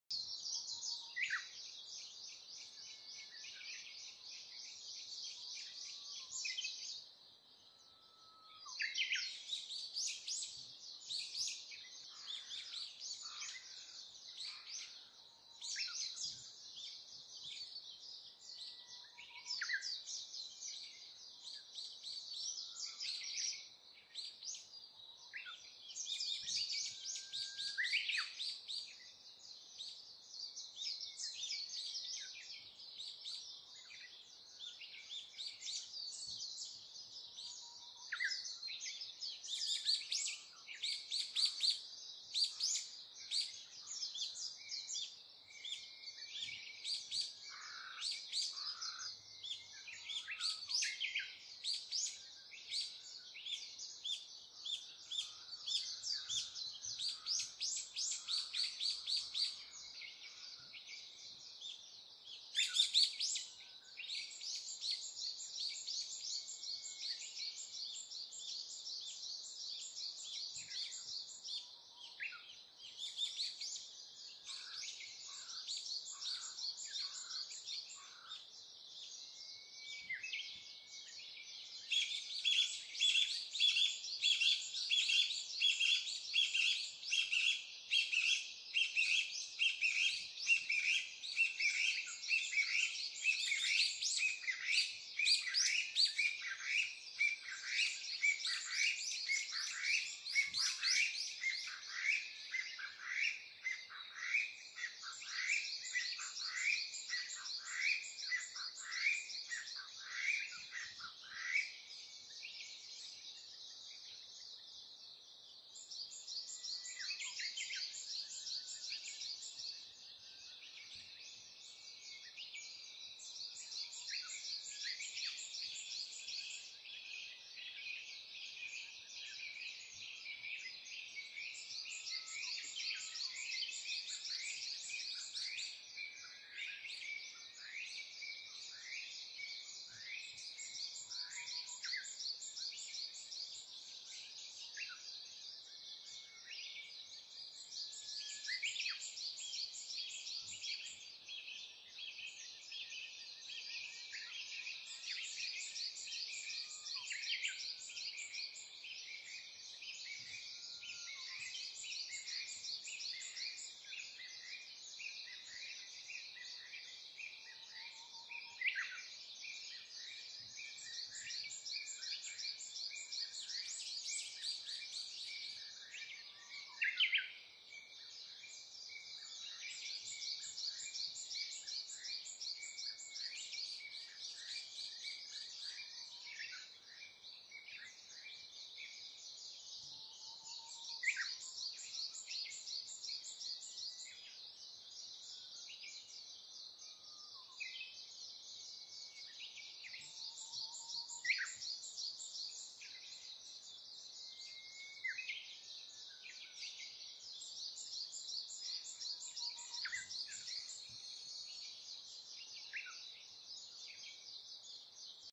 Warbling white-eye & Brown-eared Bulbul Eating Orange
0:12 Warbling white-eye
1:15 Brown-eared Bulbul Eating Orange